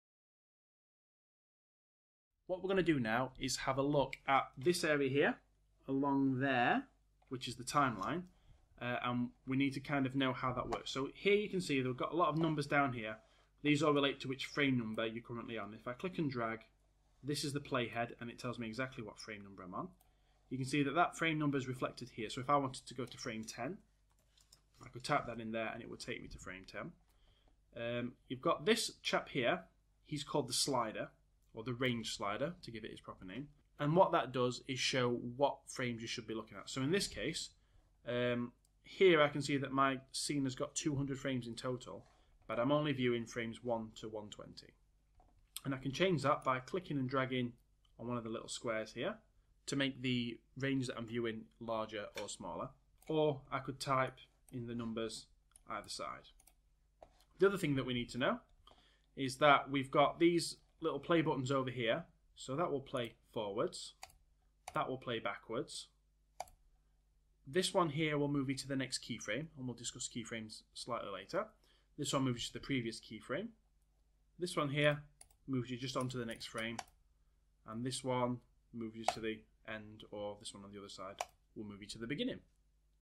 #3 Understanding the Timeline | Maya 2016 Animation tutorial
In this part of my Maya animation tutorial we will take a look at the timeline and discuss its role in creating animation using Maya 2016.

This tutorial continues directly from my Maya 2016 for beginners tutorial.
To download the project folder to allow you to work on this tutorial without completing the previous tutorial
If you did complete the previous tutorial you'll still need the image sequence for the animated texture.